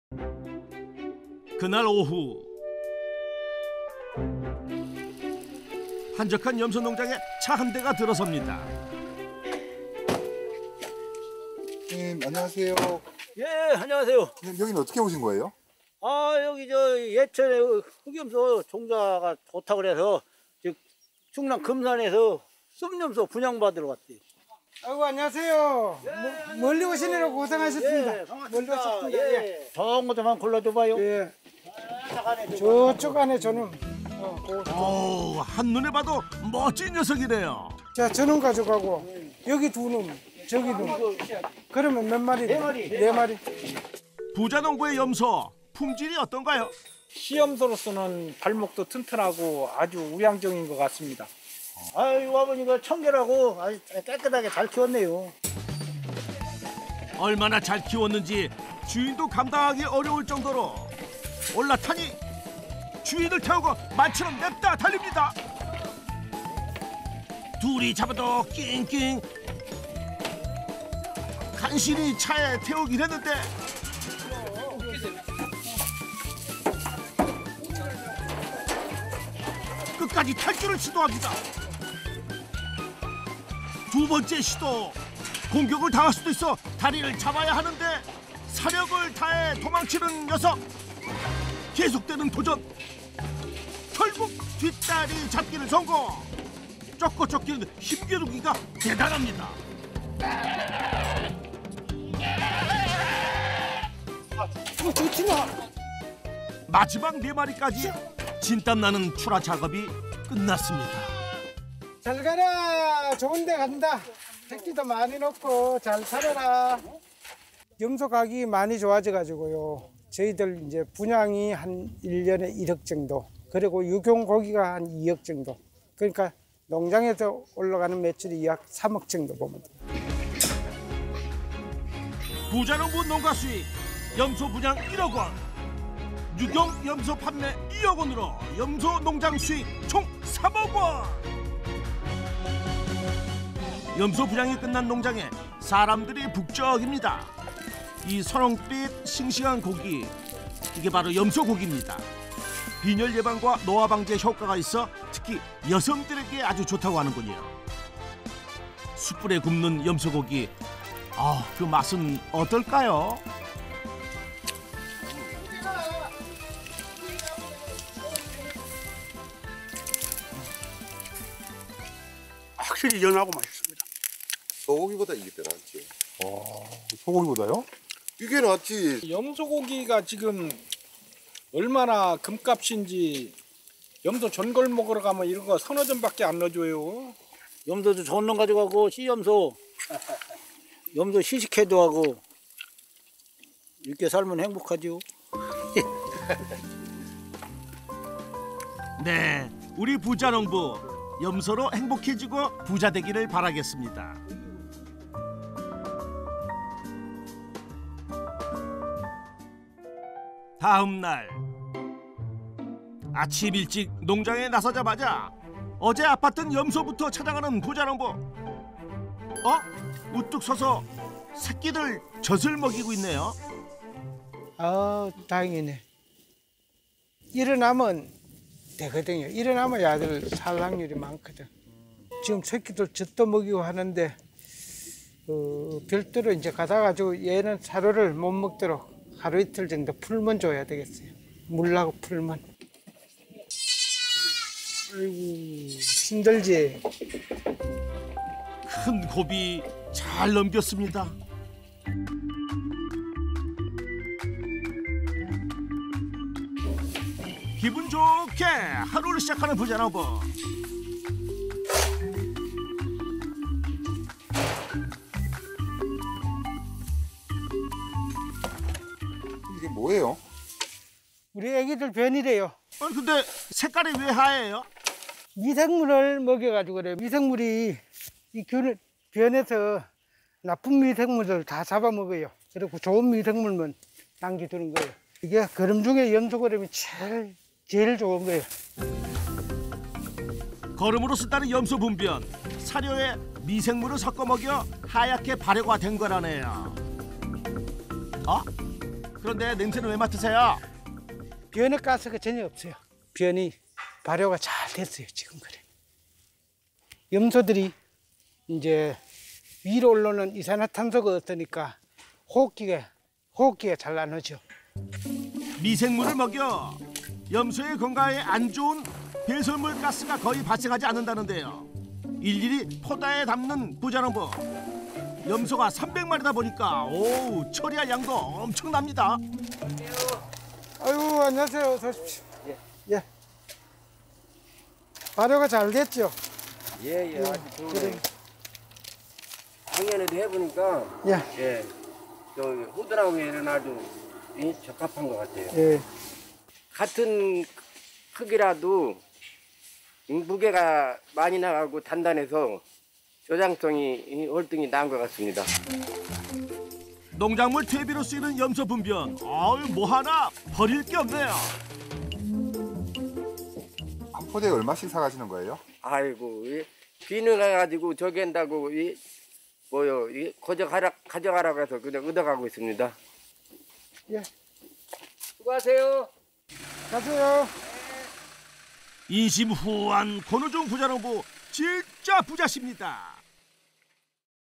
[부자농부) 염소 매력에 이끌려 시작! 연 매출 5억!!] 부자농부의 염소 품질은?
선홍빛 싱싱한 고기 이게 바로 염소고기입니다
빈혈 예방과 노화 방지에 효과가 있어
특히 여성들에게 아주 좋다고 하는군요
숯불에 굽는 염소고기 그 맛은 어떨까요

*더 많은 영상은 공식 유튜브 채널에서 보실 수 있습니다*